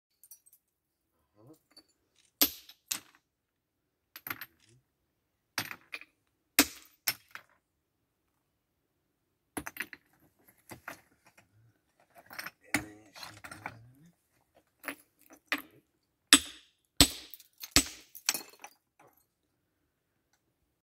V lome nad Hajnáčkou a Gemerským Jabloncom
Amatérsky mineralóg Ladislav Oravec počas hľadania zafírov v bazalte na najvýznamnejšom nálezisku týchto drahokamov v celých Karpatoch.